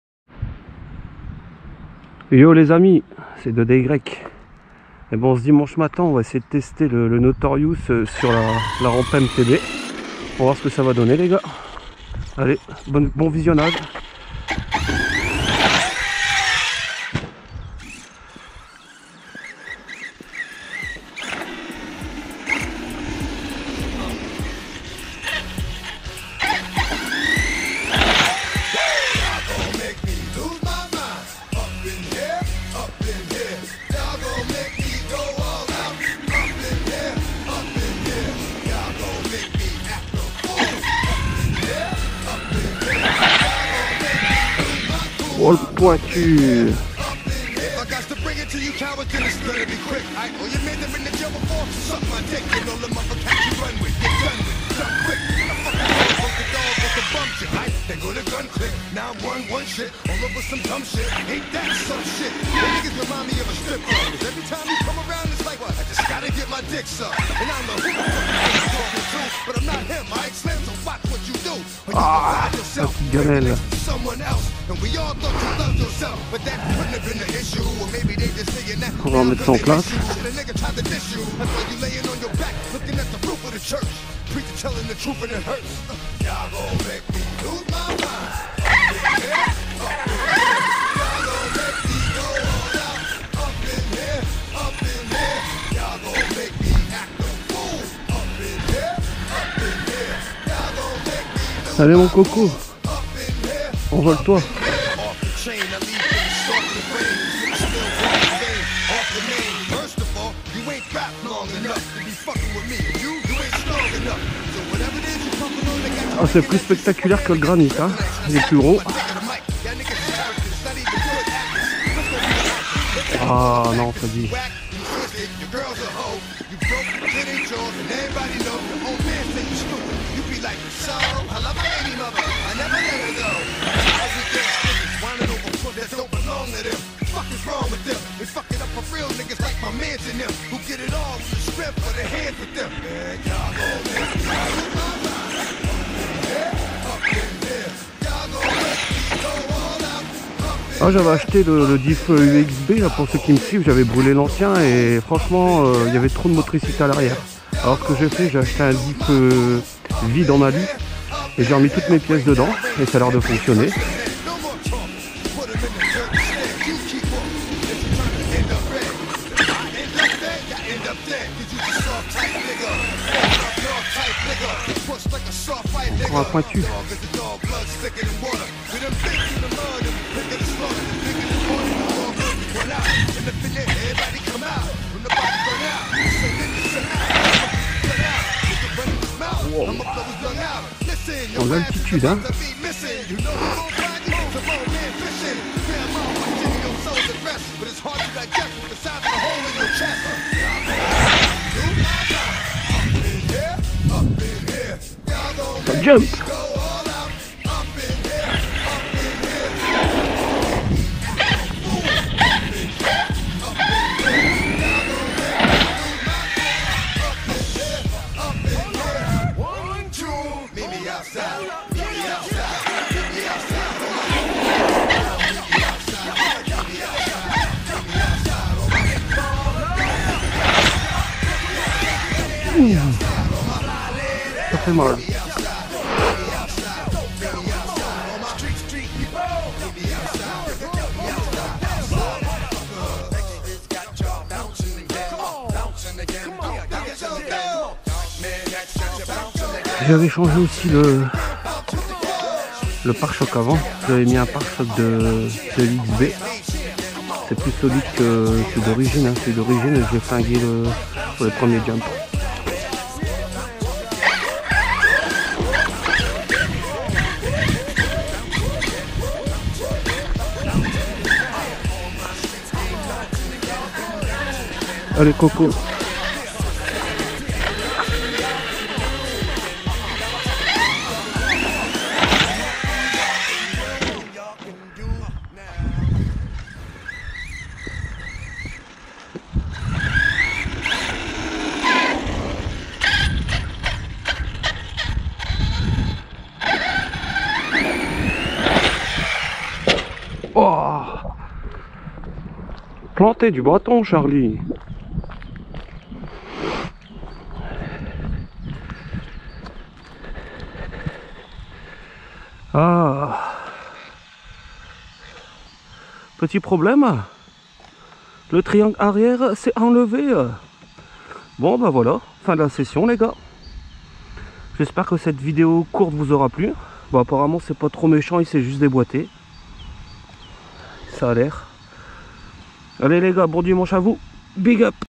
Arrma Notorious V5# Mtb Hopper Pro, triangle arrière droit déboité ♠☠
J'avais même perdu le cardan! j'ai du retourner sur le terrain , pour le retrouver.

R.I.P, DMX🙁